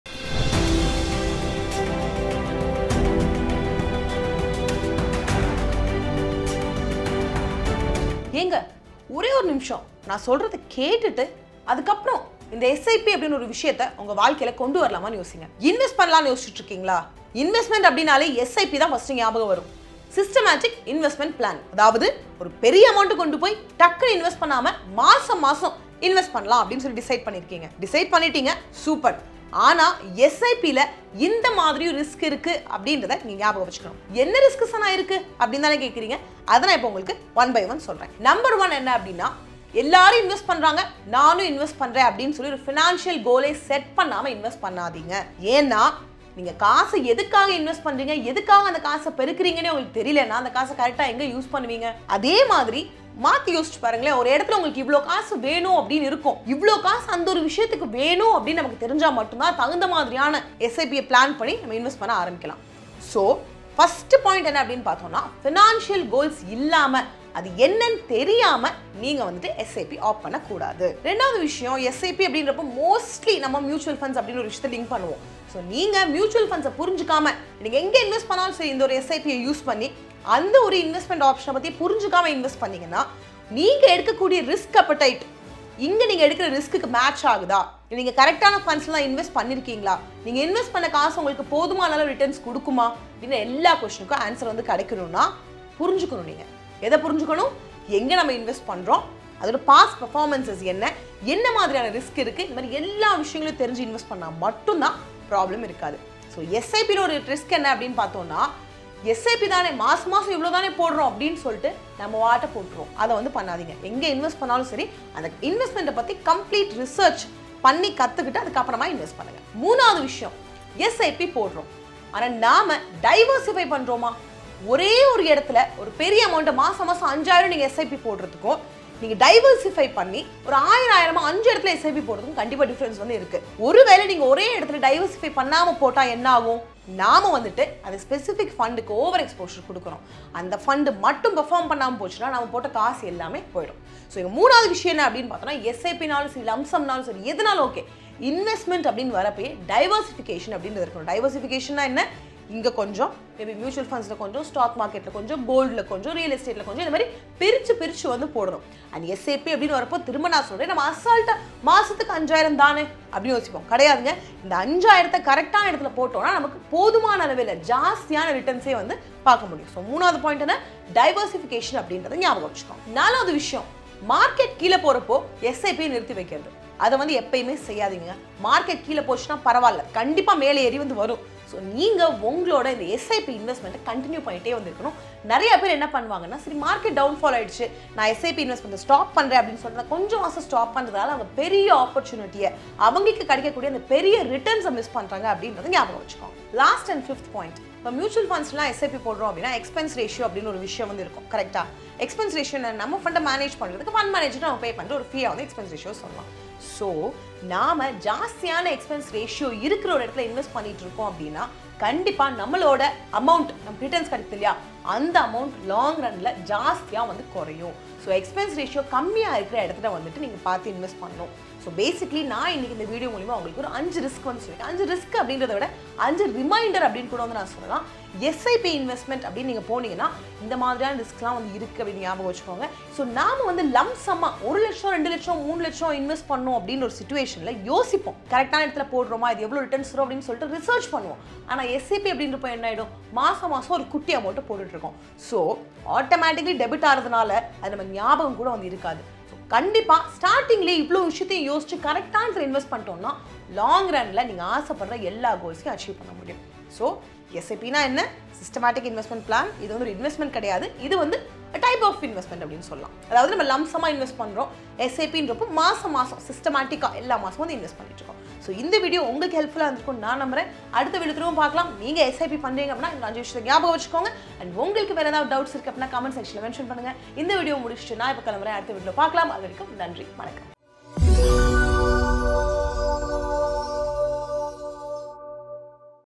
Major Risks to Avoid on SIP Investments | SIP Investment New Updates in Tamil
Systematic Investment Plan (SIP) is a great investment option for individuals looking to invest in mutual funds. However, like any investment option, SIPs also have certain risks and potential mistakes that investors should be aware of. Here are some mistakes to avoid while investing in SIPs:
Not setting financial goals: Before investing in SIPs, it is important to define your financial goals. Not setting a goal can lead to random investments, and the investor may end up not achieving the desired result.
Investing without understanding: It is important to understand the mutual fund scheme's objectives, past performance, and risk before investing in it. Not understanding these factors can lead to an investment in the wrong fund or fund that doesn't suit the investor's risk appetite.